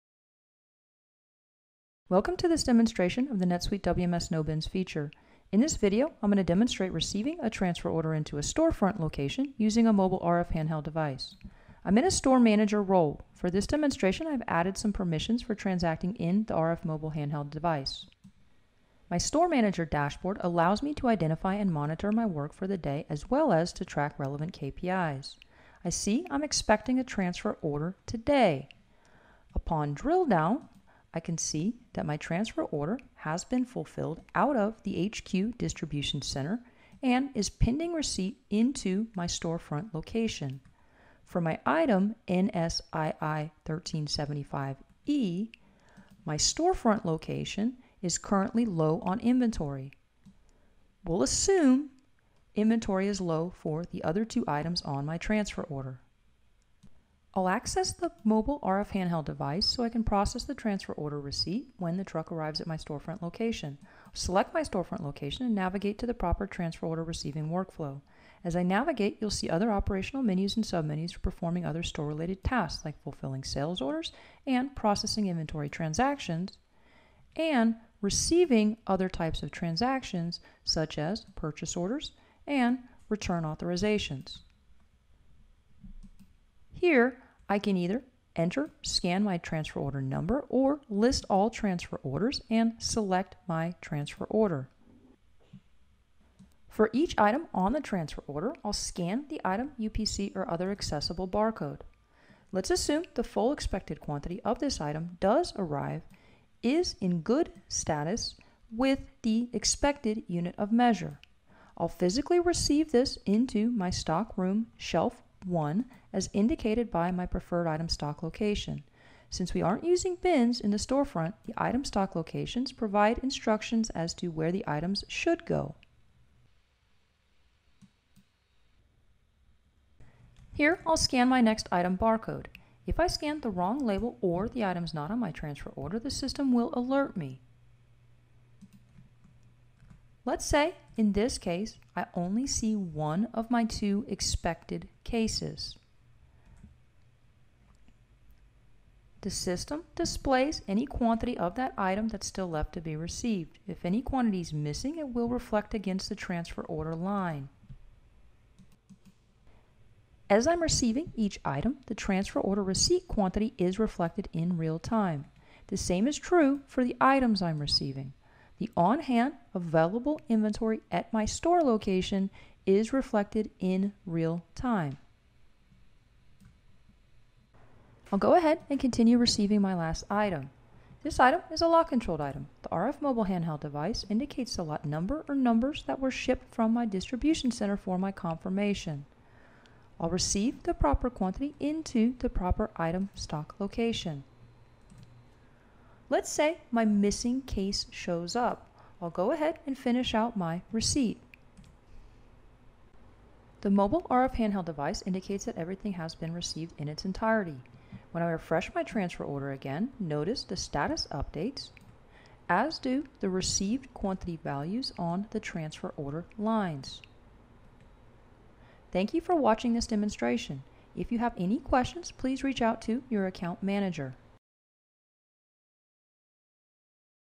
NetSuite WMS No Bins: Receiving a Transfer Order
In this demonstration, we’ll receive a transfer order using a mobile RF device with NetSuite’s Warehouse Management Solution (WMS). Starting on the dashboard,  we show how to drill-down into transfer orders to see more information, including the location from which the order is being fullfilled as well as the amount of inventory within a user’s current location. We’ll explain how to set up permissions on mobile devices so users can process transfer orders as trucks arrive, and we’ll review the final steps to process transfer orders, including: receiving items, assigning items to locations, updating the actual quantity received and logging any items that are missing or damaged.